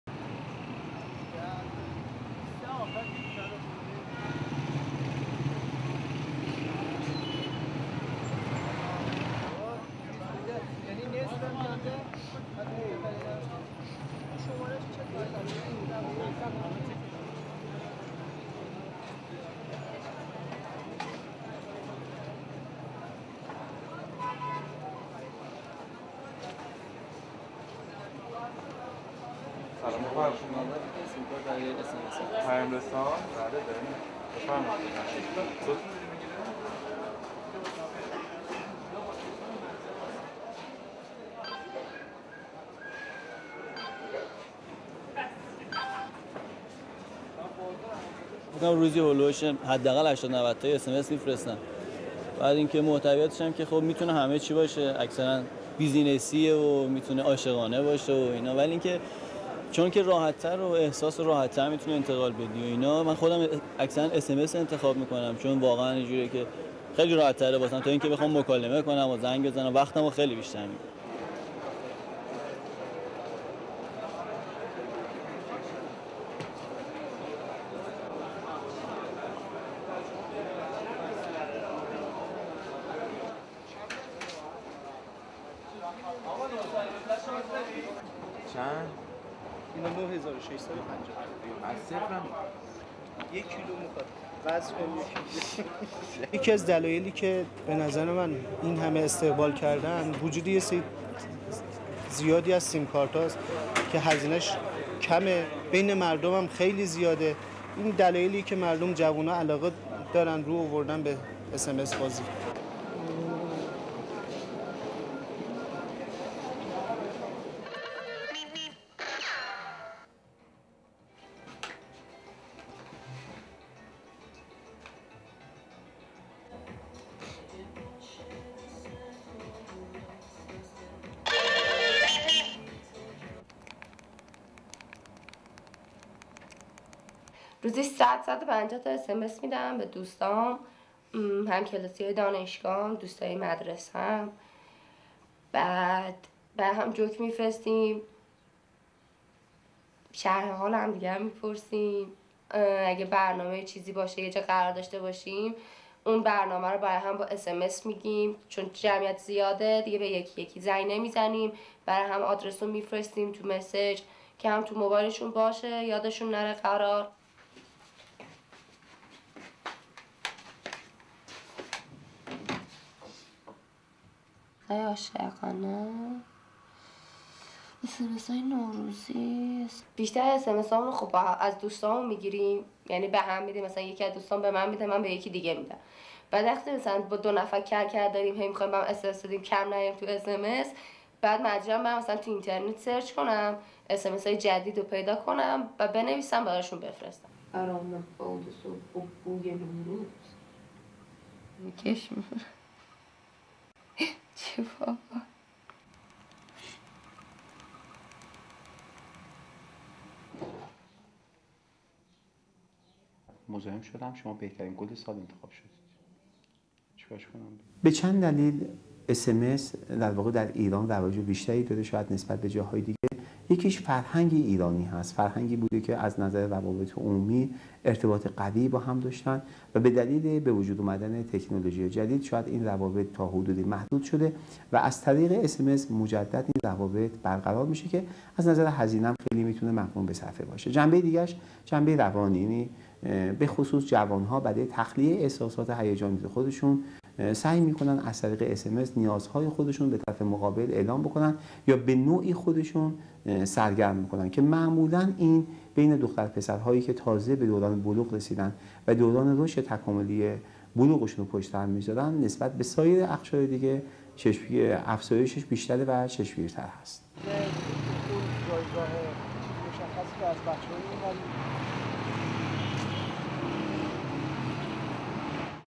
Young Iranians say it with a text
VIDEO QUALITY AS INCOMING
Tehran, Iran- February 28, 2009
1. Close up of hand sending text messages
2. Close up of face of young man smiling
3. Pan of sidewalk to two young man standing and sending text message in Tehran's Jomhouri neighbourhood, where there is a market for mobile phone and electronics
4. Pan of Tehran's Aladdin Commercial Centre
5. Close up of sign reading (Farsi) " 0919 prepaid SIM Cards and 0912 Permanent SIM Cards below the governmental price"
6. Mid of a customer buying Iran-cell carrier's special SIM card for SMS (Short Message Service) and MMS (Multimedia Messaging Service)
7. Close up of special SIM card for sending SMS and MMS
8. Wide of Tehran's Aladdin Mobile Phone Market with young man using mobile
9. Close up of handing exchanging SMS (texting)
10. SOUNDBITE: (Farsi) Ali Alavi, Frequent SMS Sender:
"I send some 80 to 90 text messages every day and the content can be everything ranging from business messages to love messages because it is easier and emotions can be conveyed better through it (SMS). I choose SMS rather than to make phone calls because it would take much more of my time."
11. Overhead shot of Aladdin Mobile Phone Market
12. Overhead shot of young man sending text message
13. Wide of people walking by and Irancell Carrier's sales booth
14. Mid of a customer discussing whole-sale price of special SMS SIM card
15. Close up of hand counting SIM cards
16. SOUNDBITE: (Farsi) Reza (no last name given), Irancell Carriers' Sales Representative in Aladdin Centre:
"The reason that people highly welcomed it (exchanging SMS) was that different carriers distributed special SIM cards and I think that is the reason people mostly the young people like it."
17. Close up of hand sending text messages
18. Close up of mobile phone displaying a text message
Tehran, Iran- March, 7, 2009
19. Wide of girl entering room and sitting down on bed with a mobile phone in her hand
20. Close up of girl smiling
21. Close up of hand rapidly sending SMS
22. SOUNDBITE: (Farsi) Sepideh Bayati, Frequent SMS Sender:
"I send some 100 to 150 texts (SMS) every day. Mostly to my friends; college friends, old friends from school. We exchanges jokes, find out about each other and what we do, if there is going to be a get-together, we let everybody know through SMS because there are too many people so we save calling everybody. SMS also goes to their inbox and they won't forget about the would-be event."
23. Wide of Sepideh Bayati entering room and sitting to search for new love messages or jokes
24. Close up of computer screen on google search
25. SOUNDBITE: (Farsi) Sepideh Bayati, Frequent SMS sender:
"I mostly rely on my friends for much of the SMS I send everyday but if I want to enter fun past time activity like record-breaking SMS exchanging with someone then I would search the web to get new SMSs. "
26. Wide of Sepideh Bayati sitting at her desk searching the web
27. Close up of the screen of the laptop displaying a funny SMS
28. Mid of Sepideh Bayati sending the SMS she searched on the web
Tehran, Iran- March 14, 2009
29. Wide of office with Dr. Mohammad Ahmadi, Sociologist, sitting at desk
30. Close up of hand checking inbox on mobile
31. SOUNDBITE: (Farsi) Dr. Mohammad Ahmadi, Sociologist and University Professor:
32. Mid of BTS antenna
33. Mid of young man exchanging SMS
LEAD IN:
Young Iranians love their mobile phones.
And their favourite way to use them is to send text messages (SMS).
STORYLINE:
Frequent texter Ali Alavi says he finds it easier to express himself by SMS.